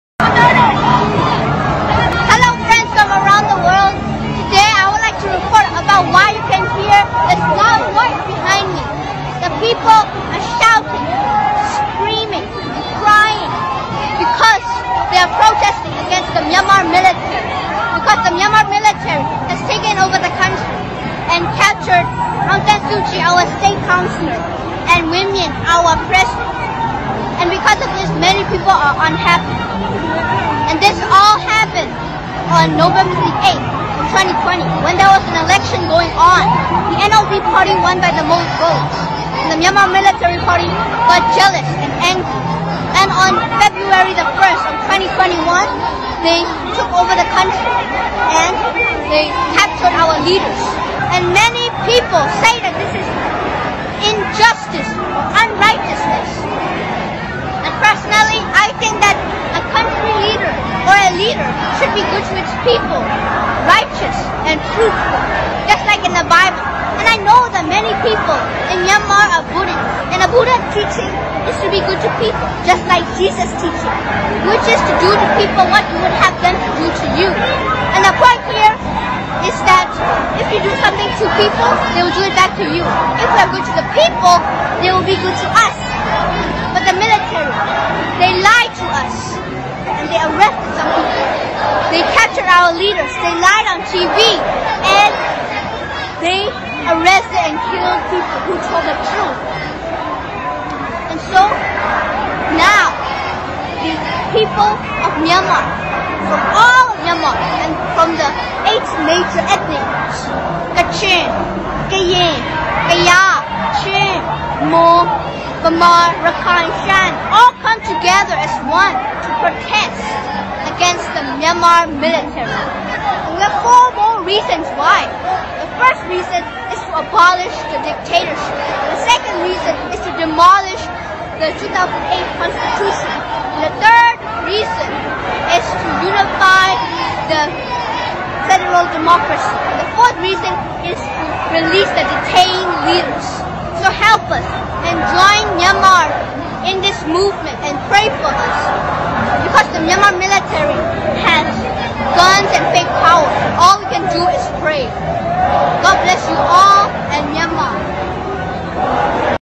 The reality of the Myanmar by a boy
This is the reality in Myanmar and how military detained Myanmar and reasons behind why.
You may see and get the reality of Myanmar from this video. Stay strong my people in Myanmar.